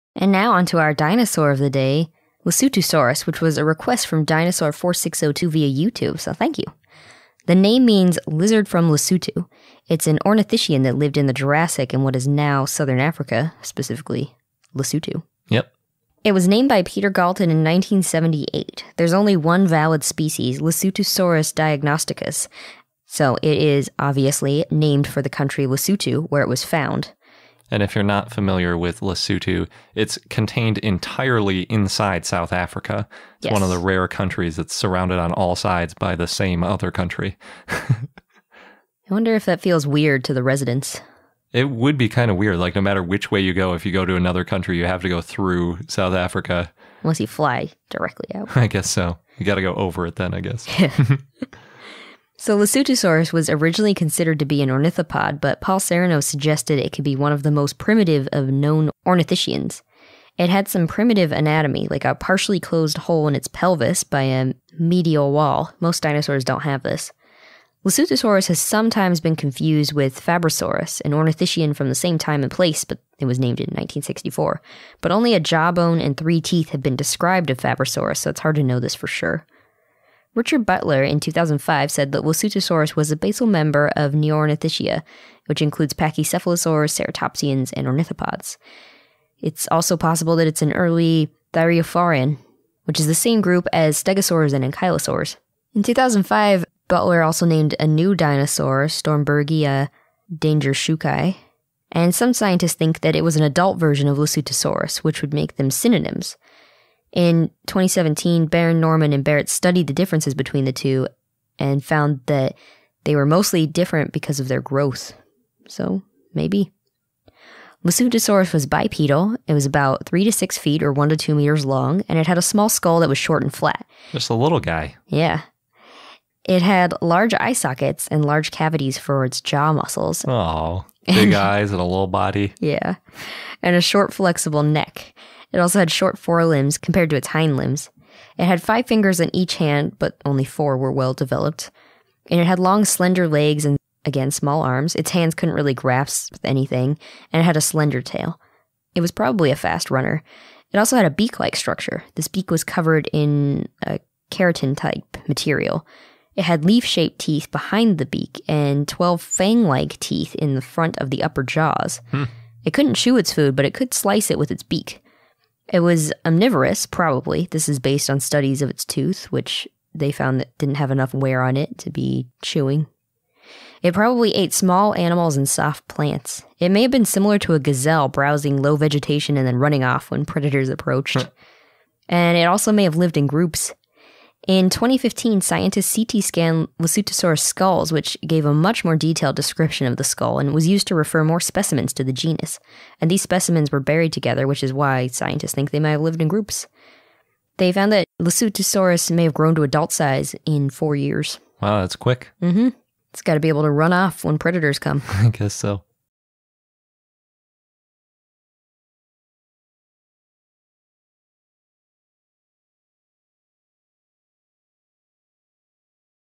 Lesothosaurus: Dinosaur of the Day
Lesothosaurus

Name means “lizard from Lesotho”
Ornithischian that lived in the Jurassic in what is now southern Africa (Lesotho)
Named by Peter Galton in 1978
Only one valid species: Lesothosaurus diagnosticus
Named for the country Lesotho, where it was found
Originally considered to be an ornithopod, but Paul Sereno suggested it could be the most primitive of known ornithischians
Had some primitive anatomy, like a partially closed hole in its pelvis by a medial wall (most dinosaurs did not have this)
Lesothosaurus has been sometimes confused with Fabrosaurus (an ornithischian from the same time and place, that was named in 1964). But only a jawbone and three teeth have been described of Fabrosaurus, so it’s hard to know for sure)
Richard Butler said in 2005 that Lesothosaurus was a basal member of Neornithischia (includes pachycephalosaurs, ceratopsians, ornithopods)
Could also be an early thyreophoran (same group as stegosaurs and ankylosaurs)
Richard Butler named a new dinosaur, Stormbergia dangershoeki, in 2005, and some scientists think it was an adult version of Lesothosaurus, which would make them synonyms. (In 2017, Baron, Norman & Barrett studied the differences between the two and found they were mostly because of their growth.)
Bipedal
About 3-6 ft (1-2 m) long
Had a small skull that was short and flat
Had large eye sockets and large cavities for the jaw muscles
Had a short, flexible neck
Had short forelimbs compared to its hindlimbs
Had five fingers on each hand (only 4 were well developed)
Had long, slender legs, small arms (hands could not really grasp things), and a slender tail
Probably was a fast runner
Had a beaklike structure
Beak was covered in keratinous material
Had leaf-shaped teeth behind the beak, and 12 fanglike teeth in front of the upper jaws
Could not chew its food, but could slice it with its beak
Omnivorous, probably (based on studies of its tooth, which found not enough wear)
Probably ate small animals and soft plants
May have been similar to a gazelle, browsing low-vegetation and running off when predators approached
May have lived in groups
In 2015, scientists CT scanned Lesotho skulls, which gave a much more detailed description of the skull and was used to refer more specimens to the genus.
These specimens were buried together, which is why scientists think they may have lived in groups
May have grown to adult size in 4 years